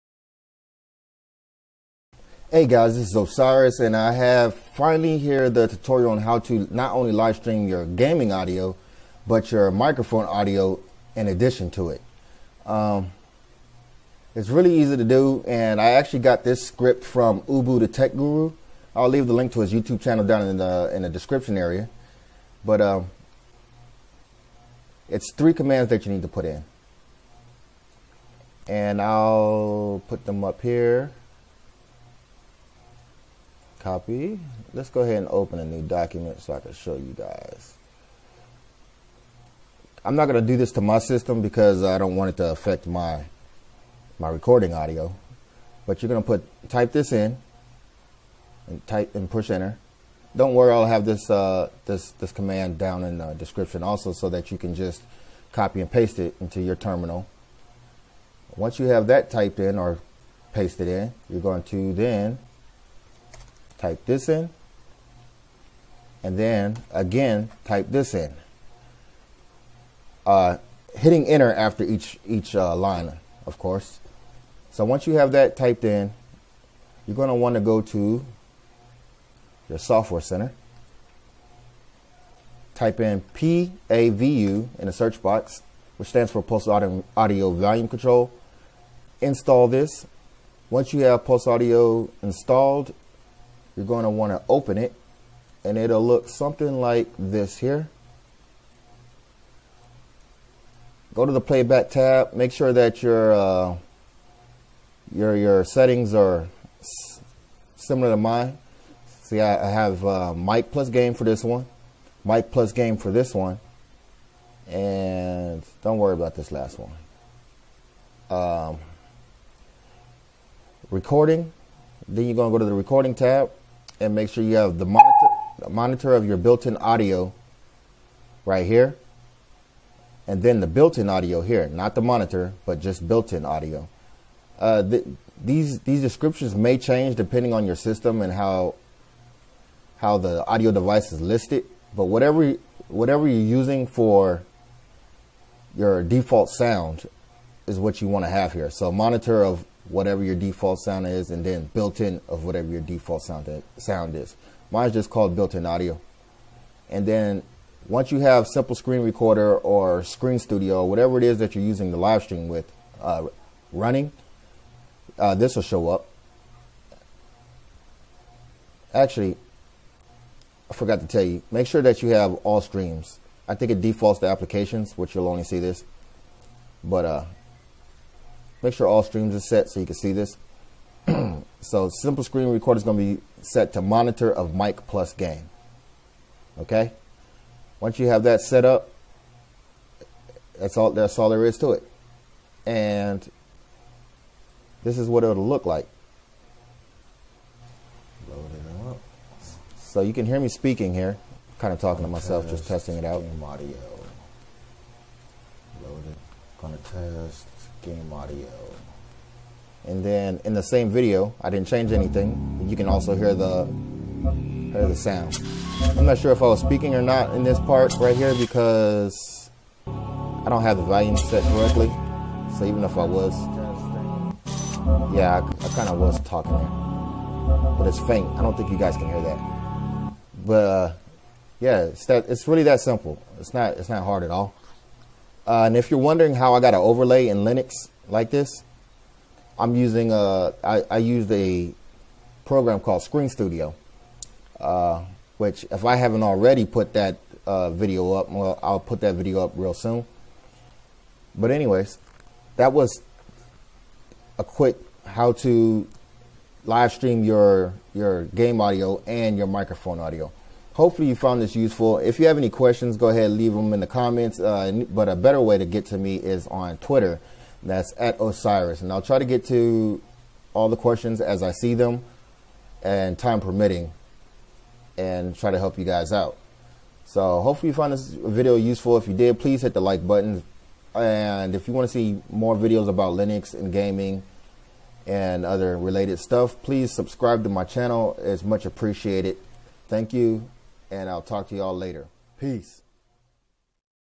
Linux Tutorial - Streaming Game & Mic Audio
Here's a quick way to stream your game and your microphone audio at the same time.

COMMANDS TO ENTER IN TERMINAL:

pactl load-module module-null-sink sink_name=virtual1 sink_properties=device.description="mic+game"

pactl load-module module-loopback sink=virtual1